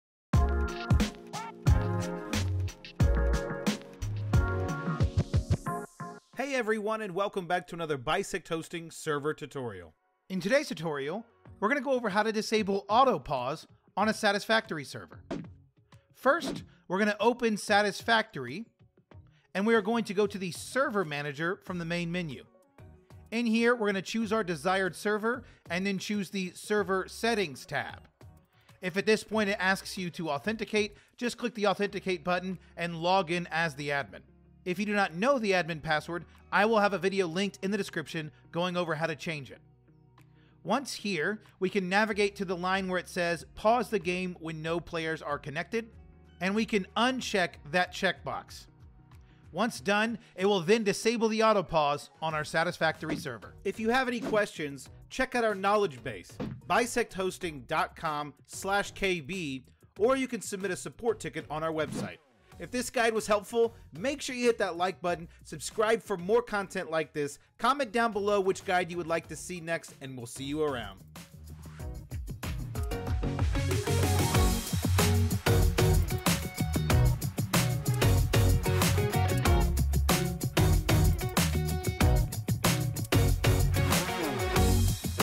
How to Disable Auto-Pause on a Satisfactory Server!
Learn How to Disable Auto Pause on a Satisfactory Server!

Disabling auto-pause allows the server to continue running when no one is logged in. This is helpful if you want your factory to continue to produce items while offline!

TIMESTAMPS:

0:00 Intro
0:15 Log Into Server
0:39 Disable Auto-Pause
0:54 Outro